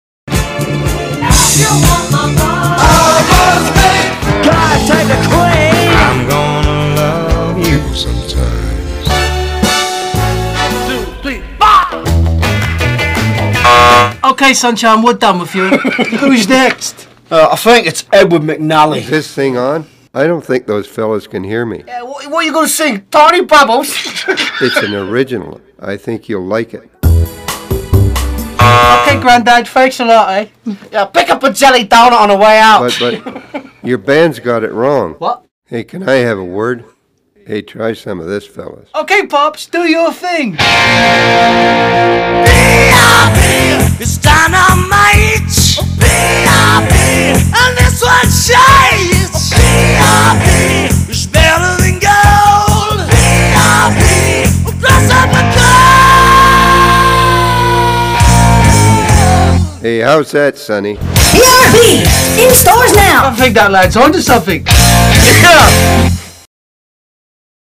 BRB
Big Rock Eddies Commercial featuring the real voice of our good friend Ed Mc Nally who passed recently. Cheers to him.